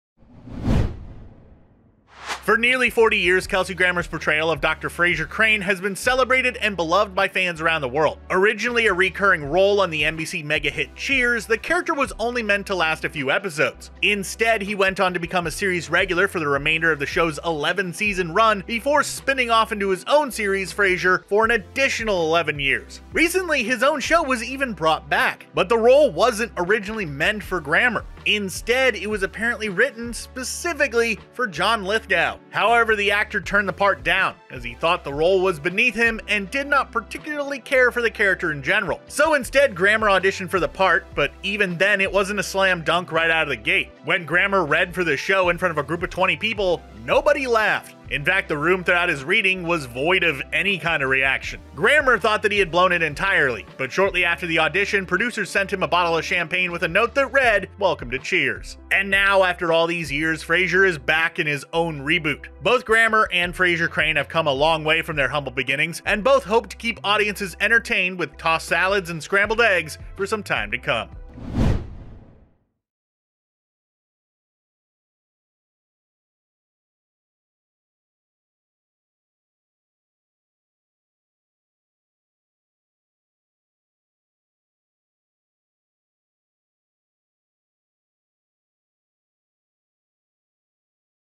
Kelsey Grammer Wasn’t the First Choice To Play Frasier Crane
For nearly 40 years, Kelsey Grammer's portrayal of Dr. Frasier Crane has been celebrated and beloved by fans around the world. Originally a reoccurring role on the NBC mega-hit Cheers, the character was only meant to last a few episodes. Instead, he went on to become a series regular for the remainder of the show's 11-season run before spinning off into his own series, Frasier, for an additional 11 years.

Recently, he was even brought back for his own series. But the role wasn’t originally meant for Grammer. Instead, it was apparently written specifically for John Lithgow. However, the actor turned the part down, as he thought the role was beneath him and did not particularly care for the character in general.

Written by: Jeffrey Harris, John Aljets
Narrated by: John Aljets  @BenderWaffles
Edited by: John Aljets